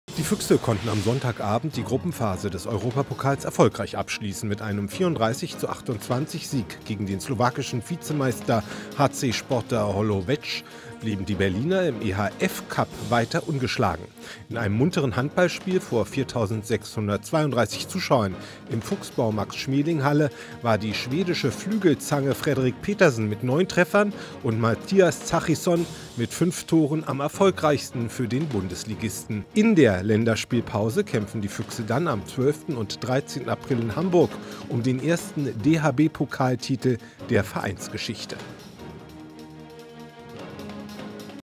Erfolgreiche Füchse
Die Füchse konnten am Sonntagabend die Gruppenphase des Europapokals erfolgreich abschließen. Mit einem 34:28 Sieg gegen den slowakischen Vizemeister HC Sporta Hlohovec blieben die Berliner  im EHF Cup weiter ungeschlagen. In einem munteren Handballspiel vor 4.632 Zuschauern im Fuchsbau Max-Schmeling-Halle war die schwedische Flügelzange Fredrik Petersen mit 9 Treffer und Mattias Zachrisson mit 5 Toren  am erfolgreichsten für den Bundesligisten. An der Länderspielpause kämpfen die Füchse dann am 12 und 13 April in Hamburg um den ersten DHB-Pokal Titel der Vereinsgeschichte.